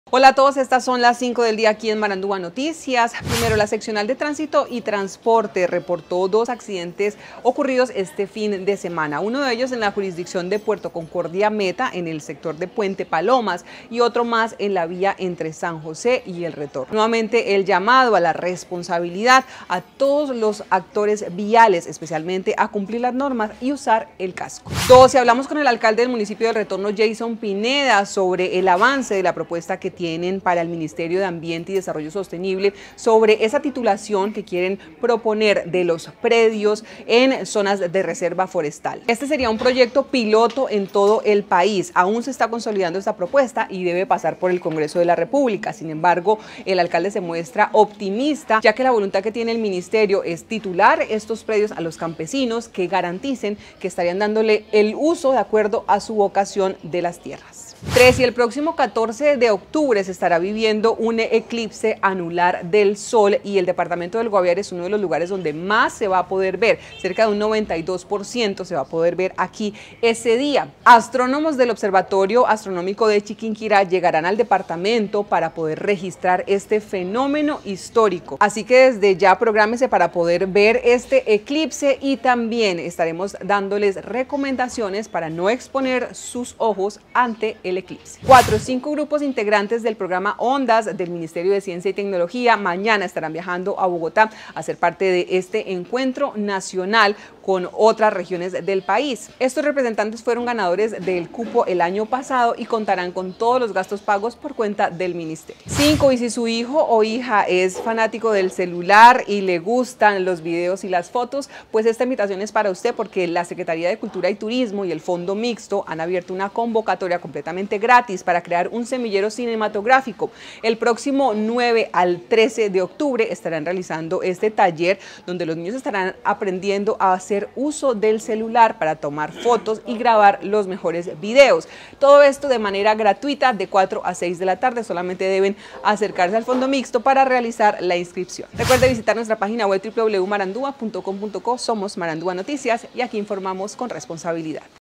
Las cinco del día en Marandua Stereo 100.7 Fm
Las cinco del día en Marandua Stereo 100.7 Fm con Laura Alvarado.

1. Dos accidentes de tránsito reportados este fin de semana
2. Campesinos de El Retorno trabajan en proyecto piloto para titulación de tierras con MinAmbiente
3. Eclipse anular del sol se verá un 91% en Guaviare el próximo 14 de octubre
4. Grupos de investigación del Guaviare participarán en Encuentro Nacional del Programa Ondas de MinCiencias
5. Abren convocatoria gratuita para Taller de creación con celular. Habrá estímulos para 32 participantes